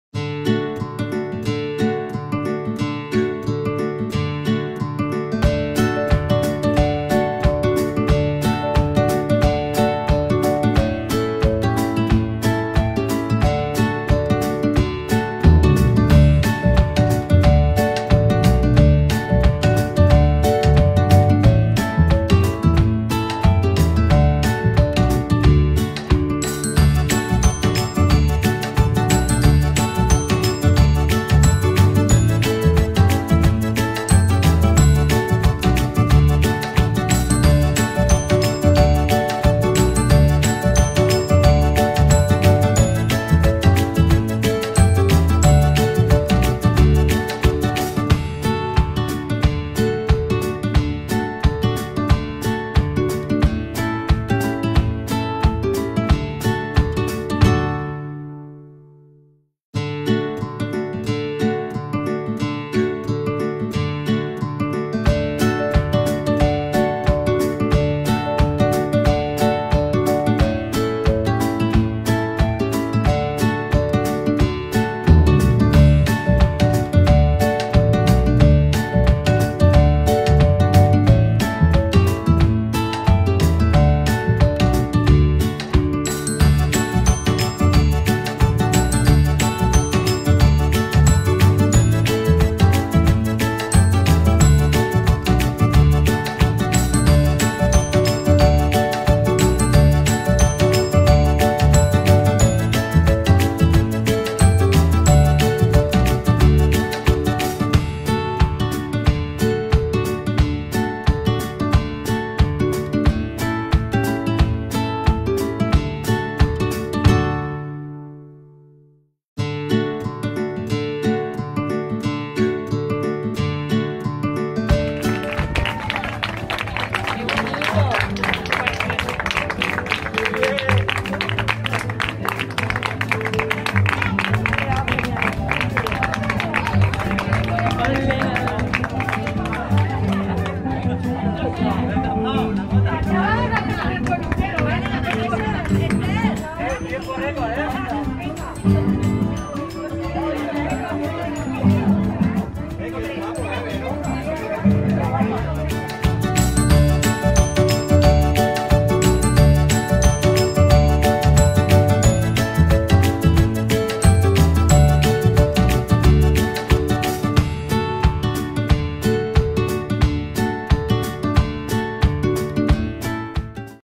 “Dame colores” mural colaborativo 2020
Mural colaborativo que se realizó en sept 2020  y está situado en la pared del parque de la cooperativa, de Losa del Obispo, Valencia.